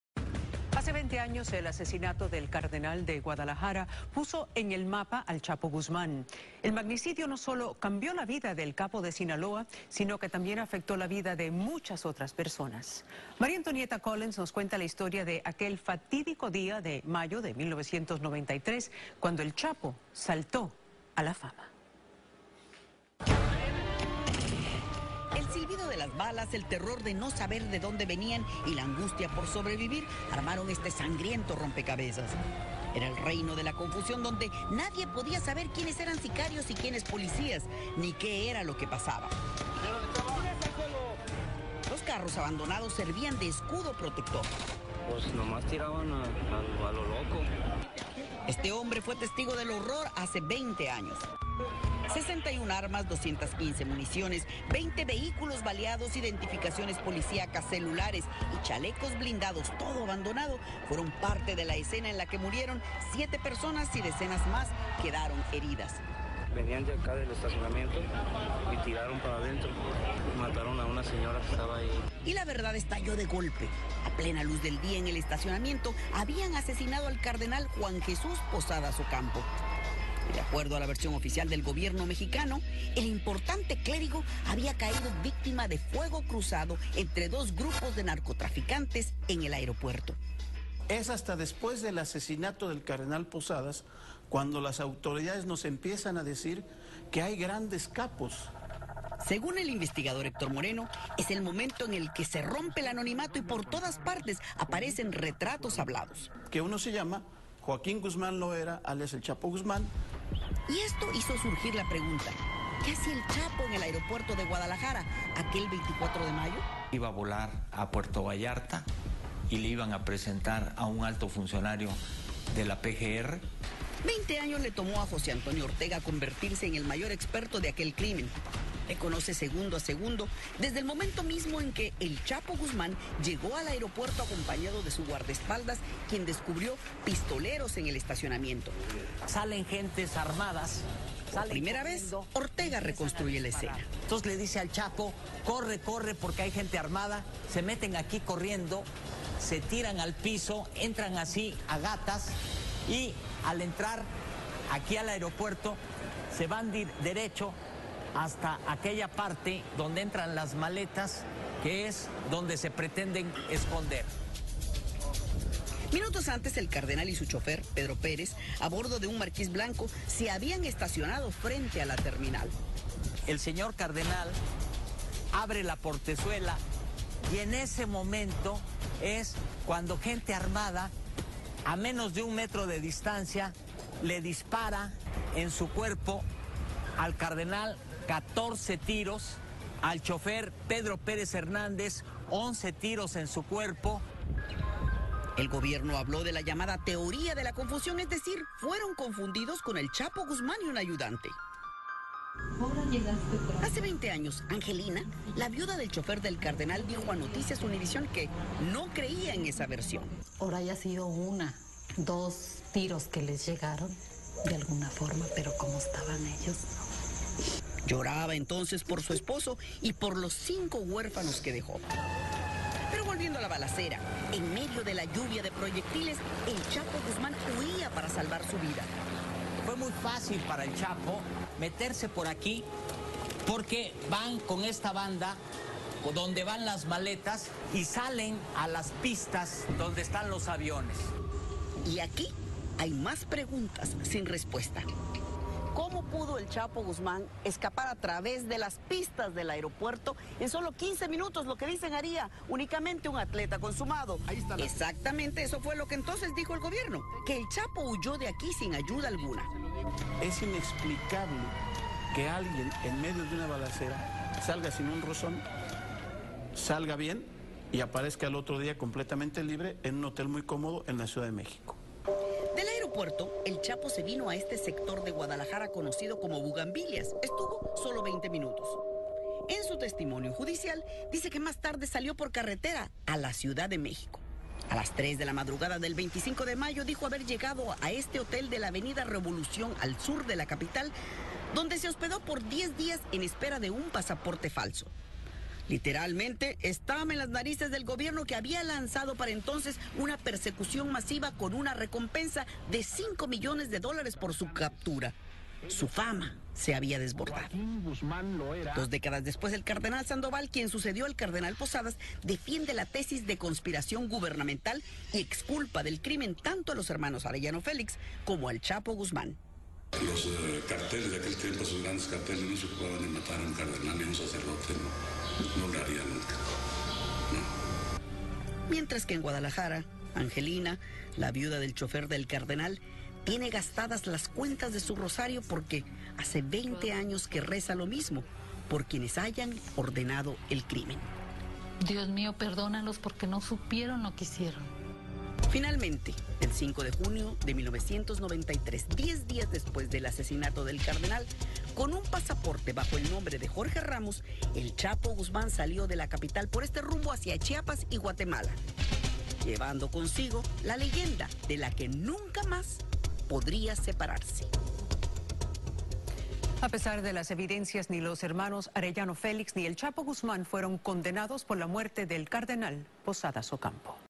El asesinato que cambió la vida a 'El Chapo' Guzmán
El asesinato del cardenal Juan Jesús Posadas Ocampo en una balacera en el Aeropuerto Internacional de Guadalajara, México, el 24 de Mayo de 1993, es una acontecimiento íntimamente vinculado a la historia de El Chapo Guzmán.

A partir de la muerte de Posadas Ocampo El Chapo pasó de operar como un narcotraficante de bajo perfil, a convertirse en el hombre más buscado de México. Cortesía de: Univisión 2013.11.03